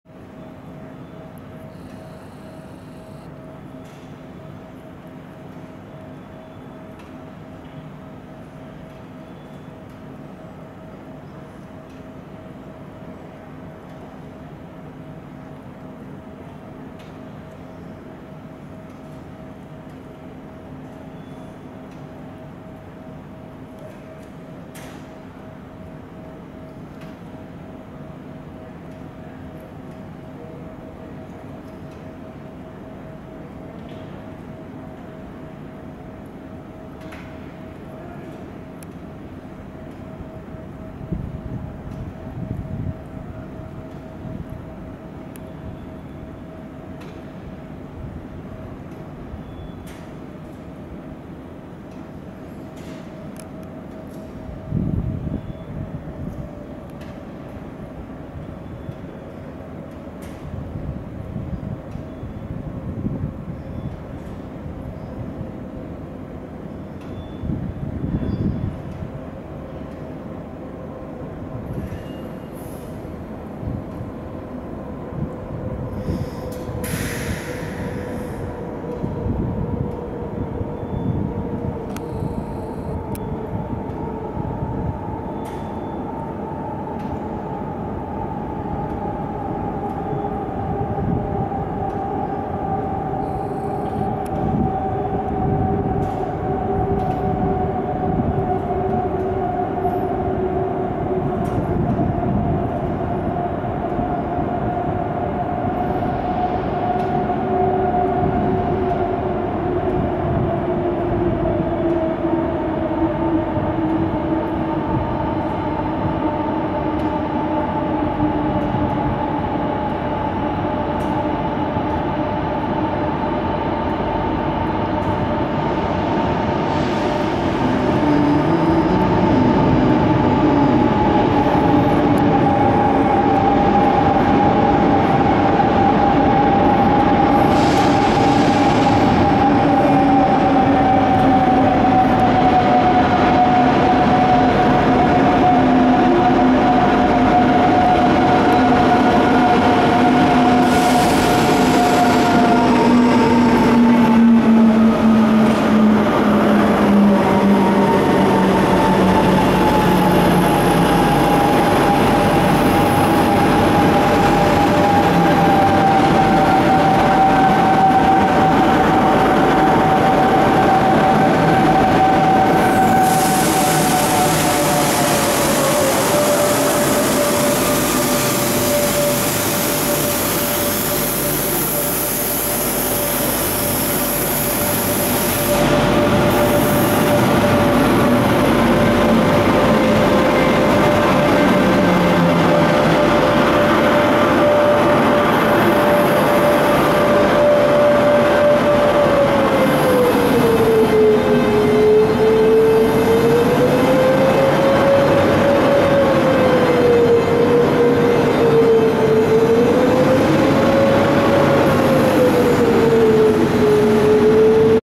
18662 @ Lisboa-Rossio
O comboio suburbano 18662, procedente de Mira-Sintra Meleças, a passar pelo túnel do Rossio, terminando o serviço na Linha 3 na estação, 13-Jul-22, 09:33.

The commuting train 18662, coming from Mira-Sintra Meleças, passing through the Rossio tunnel, ending the service on Line 3 of the station, 13-Jul-22, 09:33.